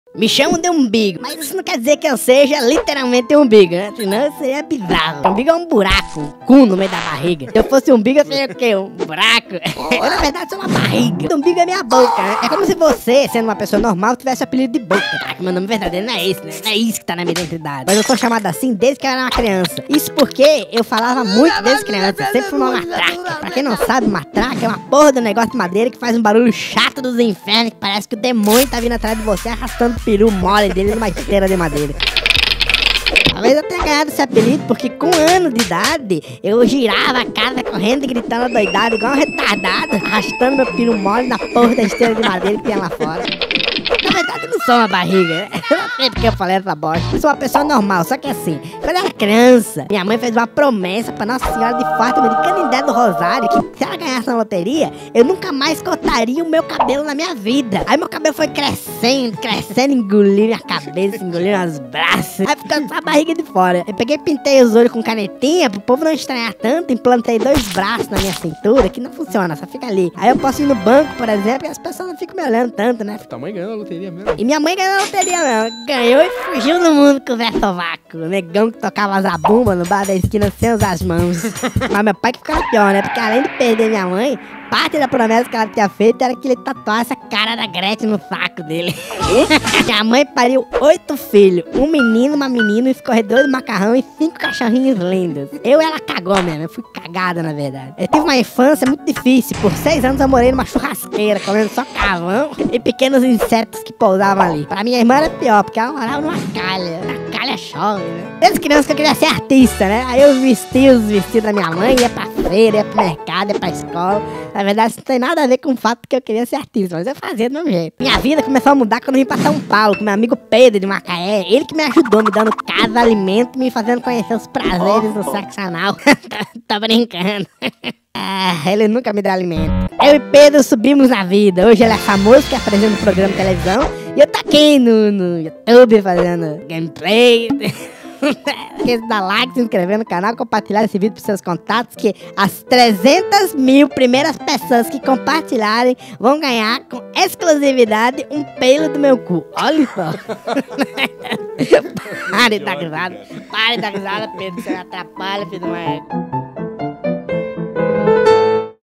O Umbigo é o Cu da Barriga - Nonsense
UM NOVO TIPO DE EDIÇÃO DORGAS!!
Estreando um novo formato de vídeo, que vai ser intercalado com os vídeos de gameplay.

Esse é tipo o meu Draw My Life zoeira/zueira/zuera!!!

UMBIGO ZUEIRO É HUMOR CRIATIVO, É HUMOR INTELIGENTE, É HUMOR HU3 MAIS COMÉDIA DO ENGRAÇADO BRASIL.

O VÍDEO MAIS ENGRAÇADO DO MUNDO, ASSUME AÍ!! HUE HUE